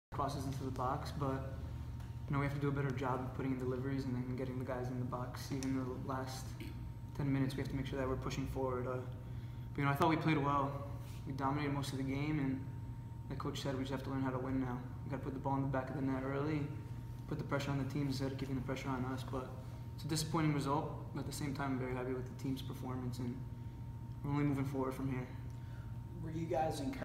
Niko Petridis Postgame - 9/2/17
Huskies fall to No. 9 Notre Dame, 1-0, on late goal by the Irish. Sophomore M Niko Petridis talks about the game.